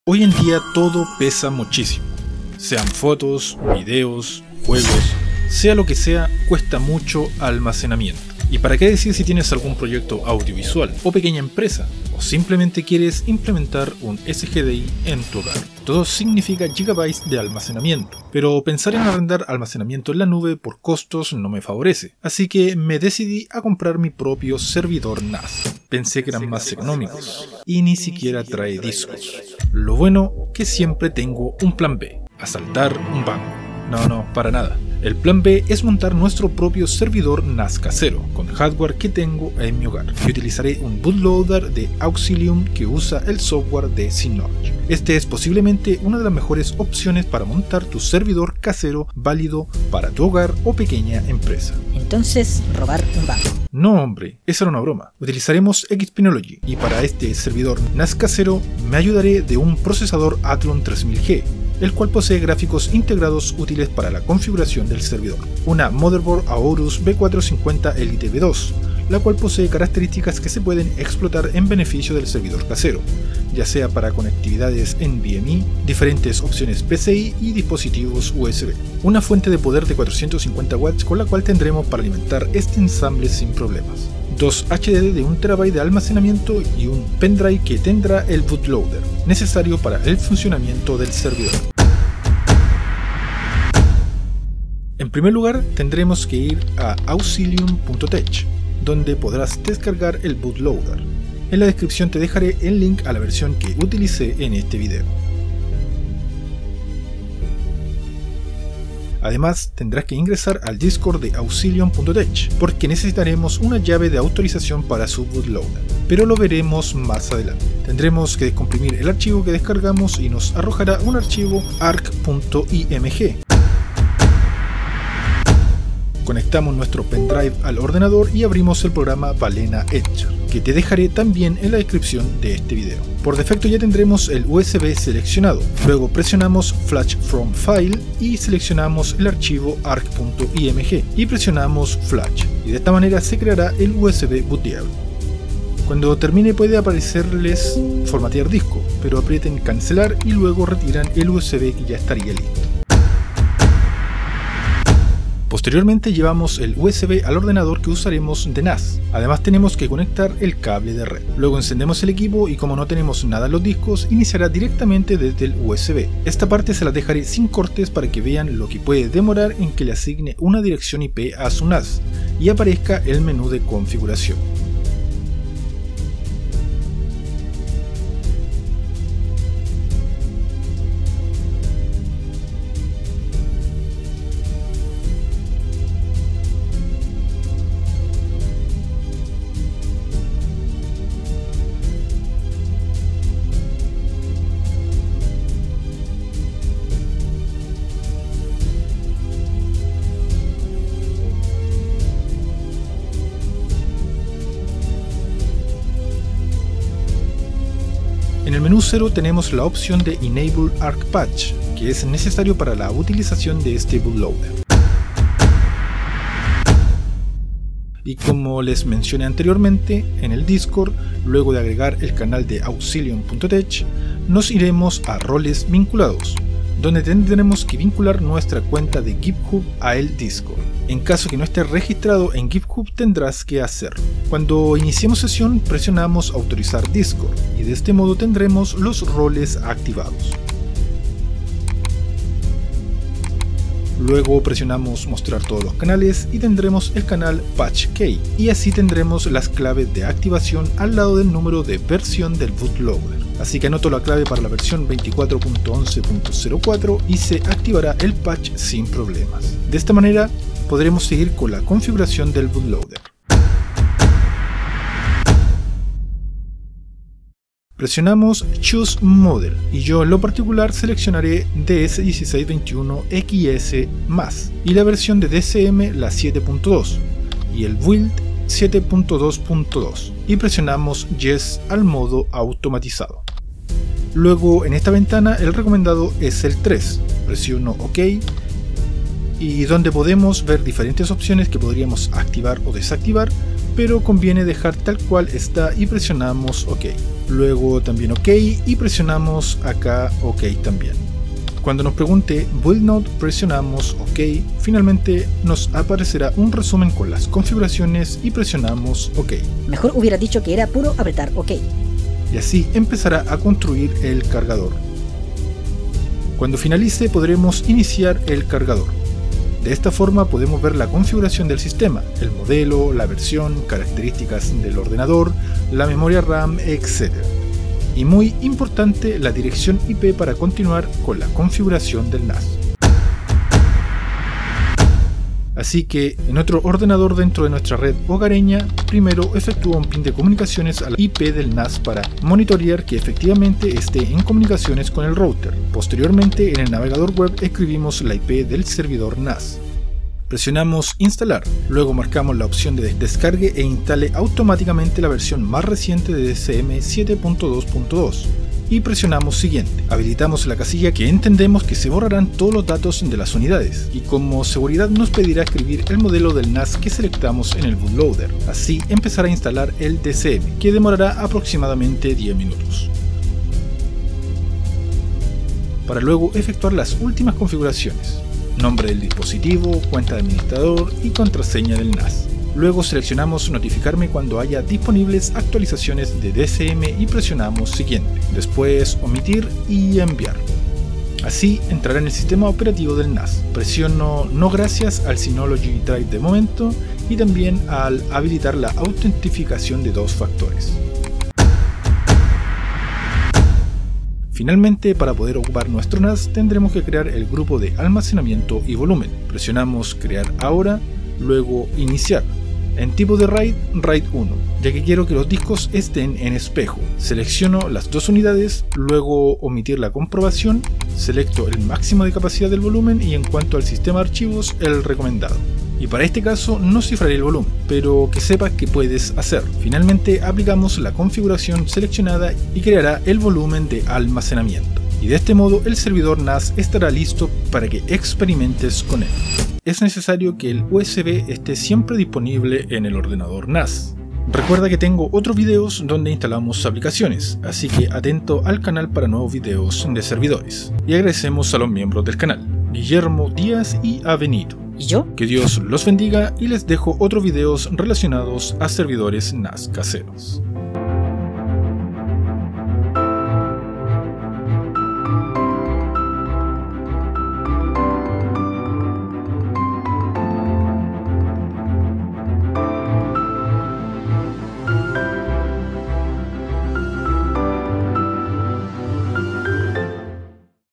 🔥Montar SERVIDOR NAS Casero - barato con Xpenology - NAS CASERO Synology DSM 7.2 - HOME SERVER
👉🚨Hoy en día todo pesa muchísimo, sean fotos, videos, juegos, sea lo que sea cuesta mucho almacenamiento. Y para que decir si tienes algún proyecto audiovisual, o pequeña empresa, o simplemente quieres implementar un SGSI en tu hogar.
Todo significa GB de almacenamiento, pero pensar en arrendar almacenamiento en la nube por costos no me favorece, así que me decidí a comprar mí propio SERVIDOR NAS..🚨

👉Índice:
00:00 Porque debería  tener un NAS
00:33 Montar servidor NAS Casero.
00:56 Hardware NAS Casero.
01:31  Auxxxilium-Tech Bootloader
02:03  Creación de USB Booteable para NAS.
02:32 Preparar NAS Casero para Instalación.
03:34 Activación ARC-Loader
04:23 Configurar Bootloader.
05:02 Instalar DSM en NAS Casero.
06:41 Crear Volumen y almacenamiento.
07:19 Recomendaciones NAS Casero.